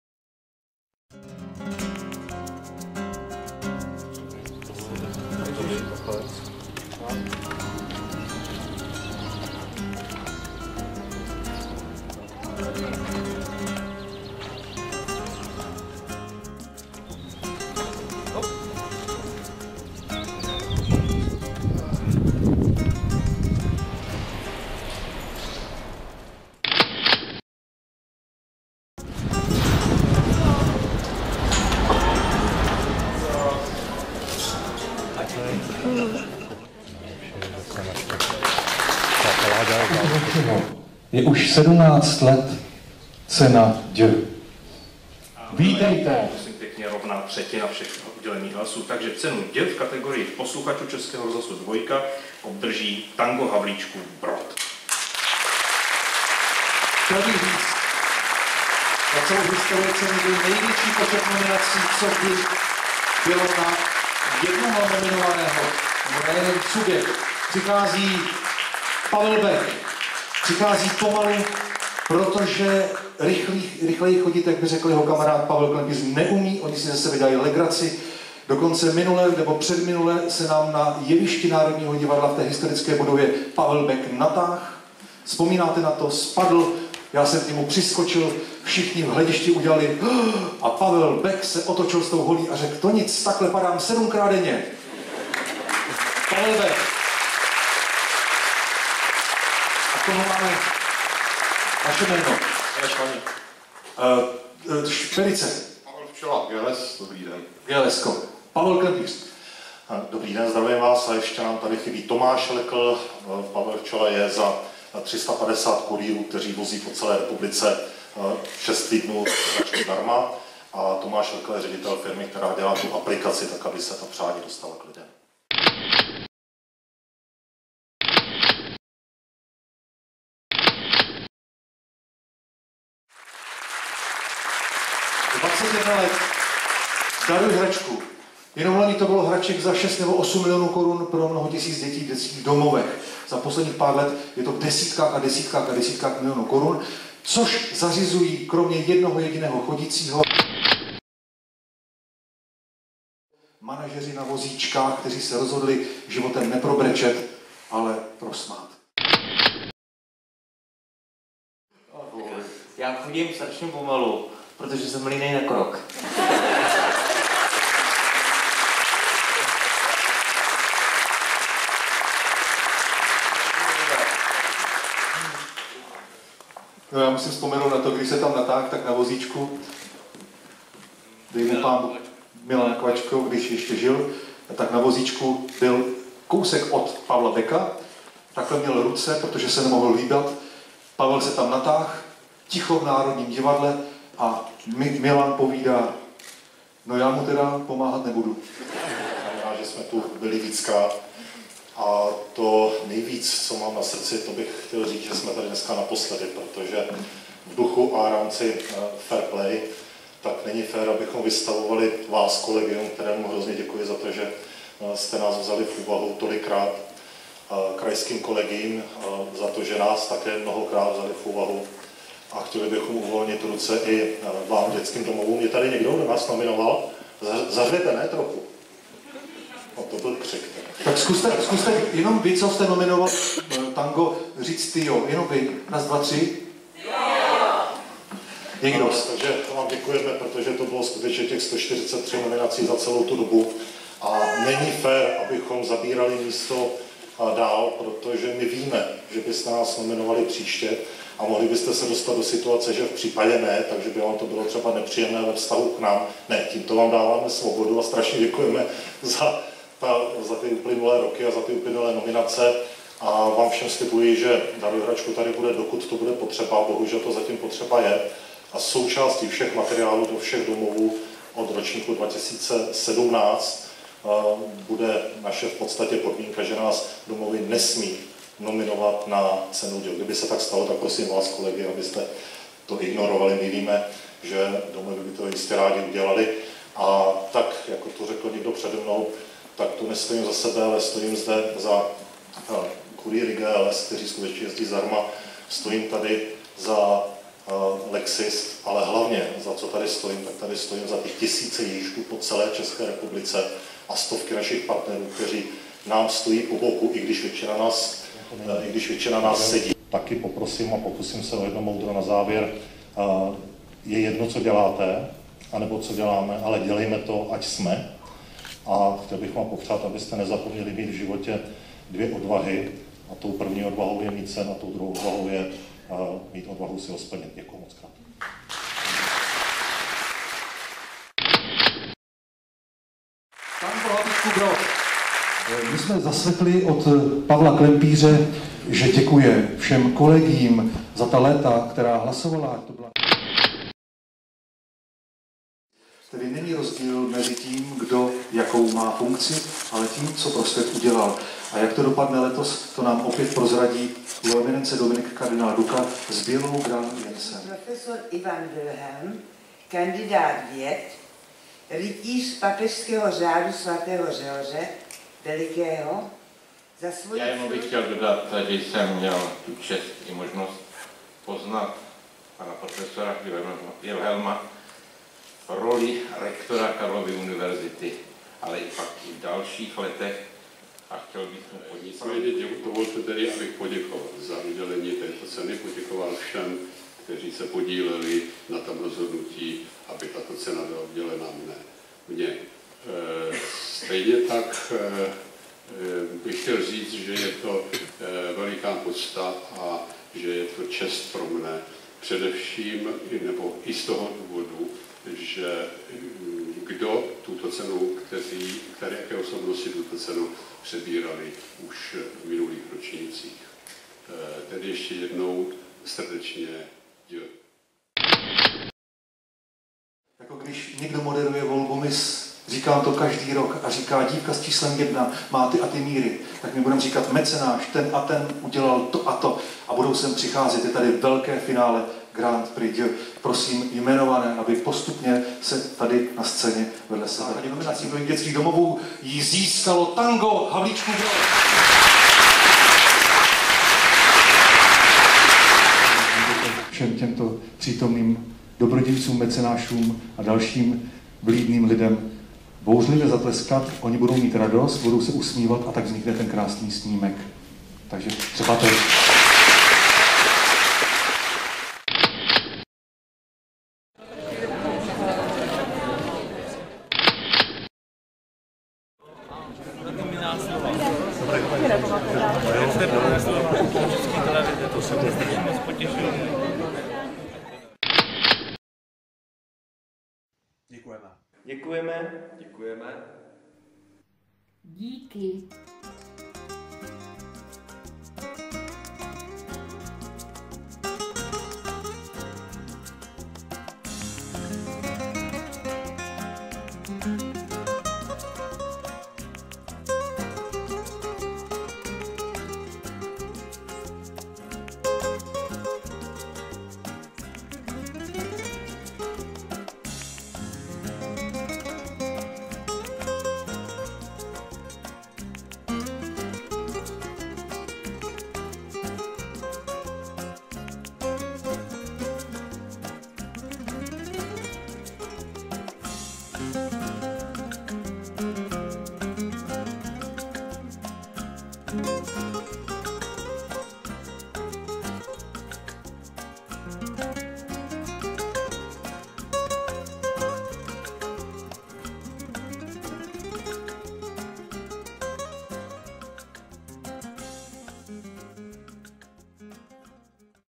Daruj hracku Tango manazeri na voziccich - udílení Ceny Ď, podekovani-blahoprani-verejne prohlaseni
Cena-Ď pro Tango – Daruj hračku, jeho Ježíšky, partnery, studenty-dobrovolníky
Takto Tango přebíralo Cenu-Ď ne za - a pro sebe, ale také za každého Ježíška Daruj hračku, každého partnera Tanga, ZP manažerů, studenta – dobrovolníka.

V srdcích a duších jste na podiu - Nová scéna Národního divadla v Praze byli s námi - blahopřejeme – děkujeme !

Nominace byly sice napsány na nás, ale každý náš partner, Ježíšek, podporovatel, přítel, student-dobrovolník, je naší součástí a cena tak patří i jemu.

Děkujeme domovům, kteří si našli chvíli času na slova, v Ceně-Ď nevídaného historického rekordu, 143 zaslaných nominací, které nám umožnily stát (vlastně sedět) na pódiích Národního divadla v Praze, Státní opery či Nové scény ND v Praze.

Považujeme za FAIR PLAY požádat domovy, aby nás ( víme, že by tak jistě znovu učinily – a velmi si toho vážíme a děkujeme! ) již nadále nenominovaly, čímž bychom jim chtěli uvolnit prostor a dát jistou „svobodu“ v nominování dalších subjektů a lidí, kteří jim pomáhají a nominaci si jistě také zaslouží a zároveň bychom chtěli tímto, dle nás férovým krokem, předejít možnému nepříjemnému pocitu domovů vůči nám v případě, že by se jednou rozhodly využít své nominace také pro jiné, nikoliv pro nás. Nebojte – Daruj hračku tu pro Vás bude, dokud bude třeba !

Co znamená být Ježíškem či partnerem Daruj hračku?
Nejen splnit sny, přání a pomoci do života.

Dáte jim tu šanci? Číslo účtu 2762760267/0100

Pomůže každých pravidelných 50, 100, 200, 300, 500 korun – jak na to – zde